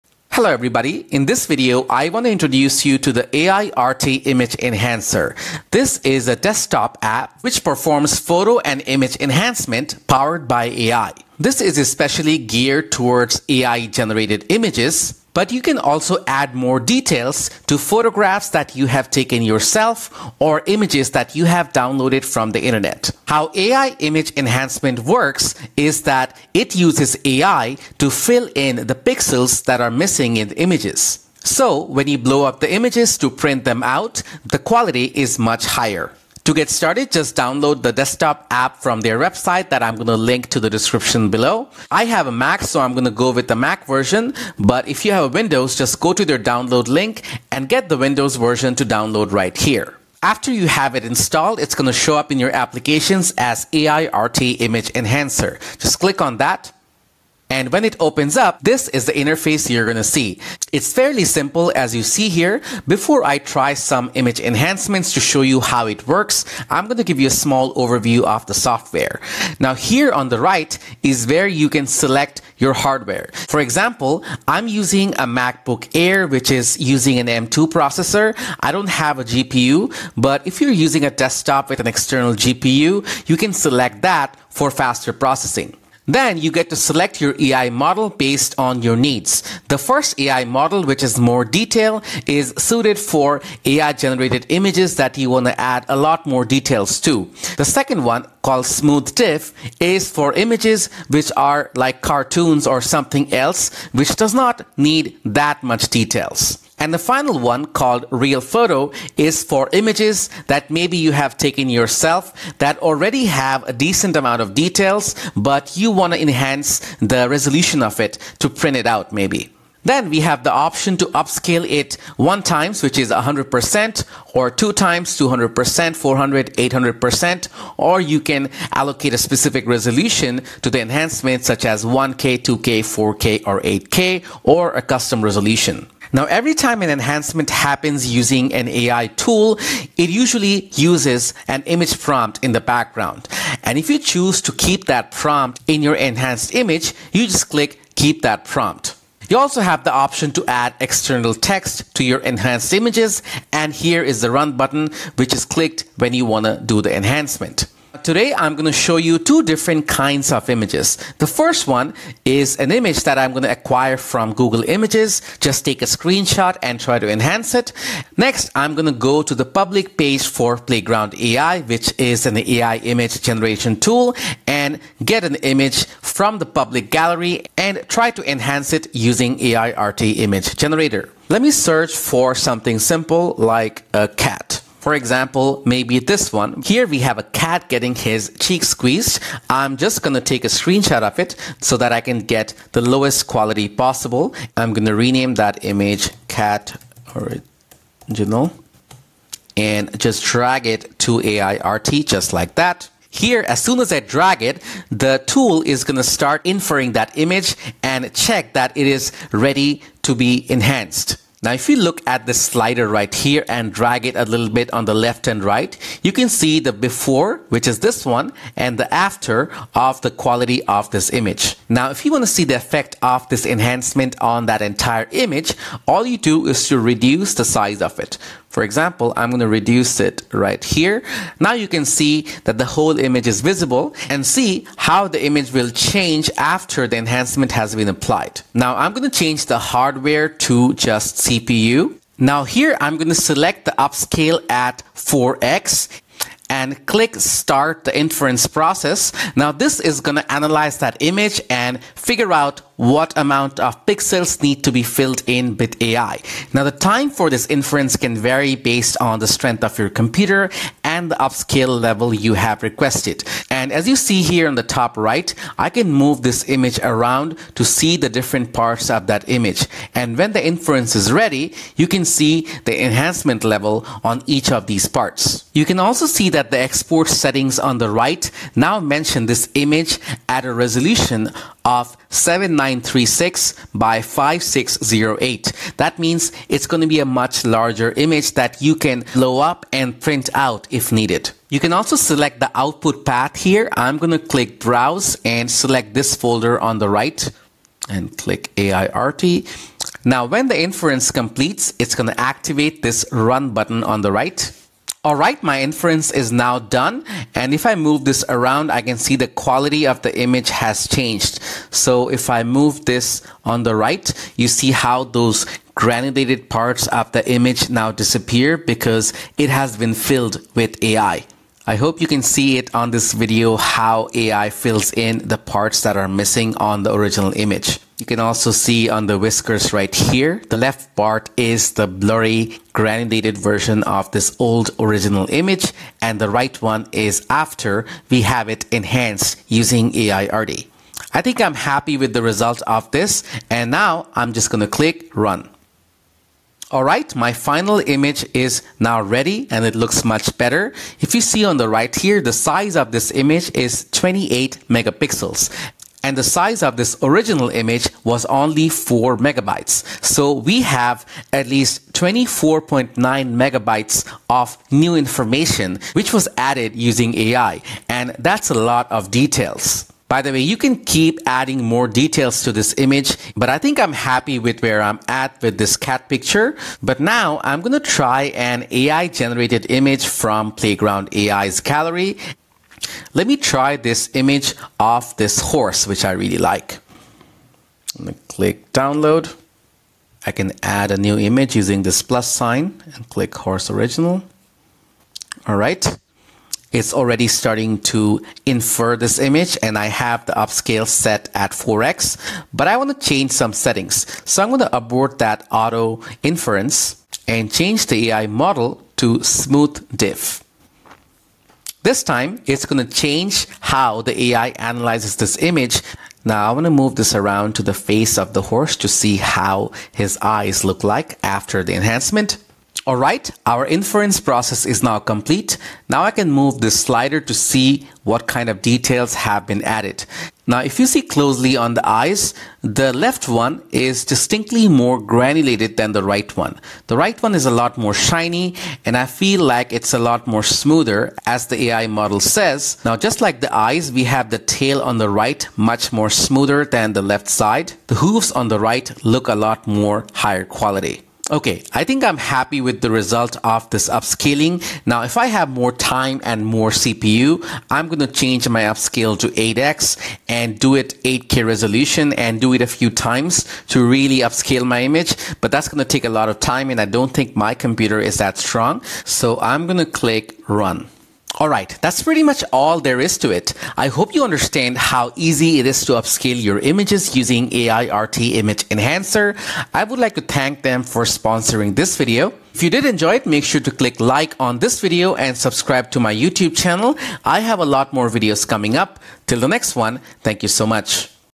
Low-Res to High-Def in Seconds: Aiarty's Image Enhancement Miracle | Best AI upscaler
Aiarty Image Enhancer is a powerful AI-powered photo and image enhancement software that can help you improve the quality of your photos in just a few clicks.

Here's what Aiarty Image Enhancer can do for you:

Enhance low-resolution, noisy, and blurry images
Upscale images to higher resolutions
Remove unwanted objects from your photos
Improve the color and clarity of your photos

In this video tutorial, I'll show you how to use Aiarty Image Enhancer to improve the quality of your photos.